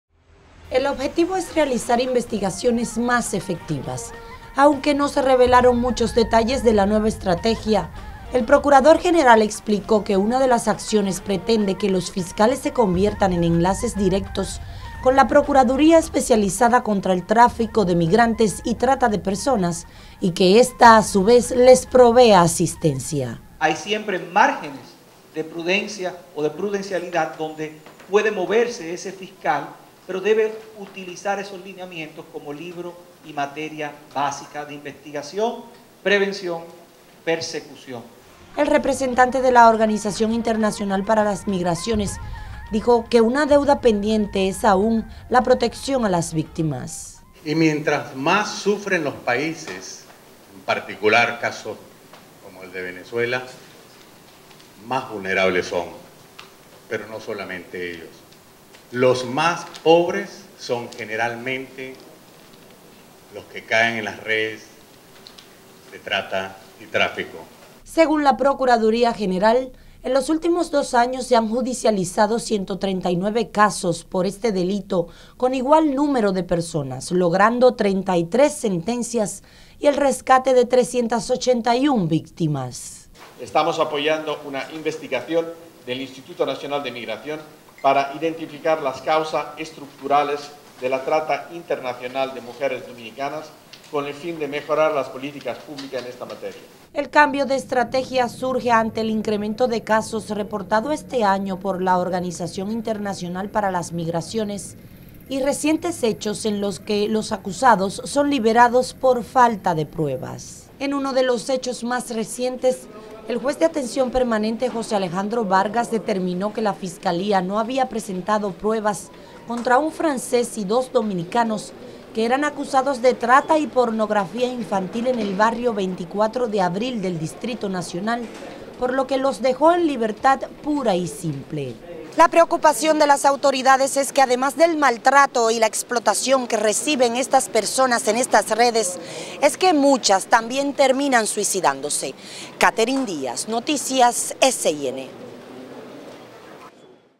PGR traza nuevos lineamientos para evitar la trata de personas
SANTO DOMINGO.- Ante el incremento este año en el país del delito de trata y tráfico de personas, reportado por la Organización Internacional para las Migraciones, la Procuraduría General implementó desde este lunes nuevos lineamientos a un de combatir este crimen.

El objetivo es realizar investigaciones más efectivas.